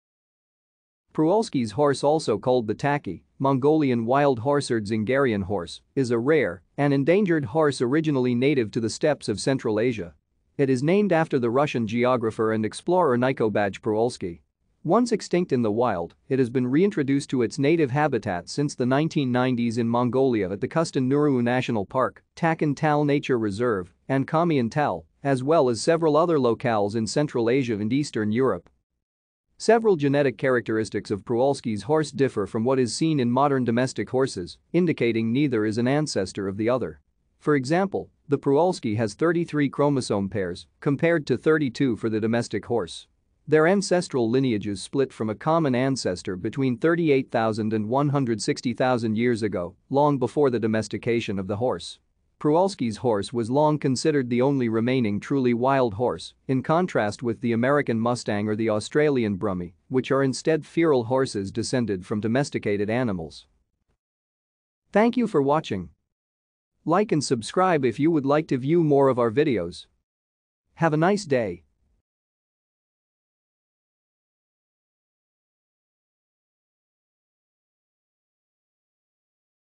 Przewalski's Horse
Przewalski's horse , also called the takhi, Mongolian wild horse or Dzungarian horse, is a rare and endangered horse originally native to the steppes of Central Asia. It is named after the Russian geographer and explorer Nikołaj Przewalski. Once extinct in the wild, it has been reintroduced to its native habitat since the 1990s in Mongolia at the Khustain Nuruu National Park, Takhin Tal Nature Reserve, and Khomiin Tal, as well as several other locales in Central Asia and Eastern Europe.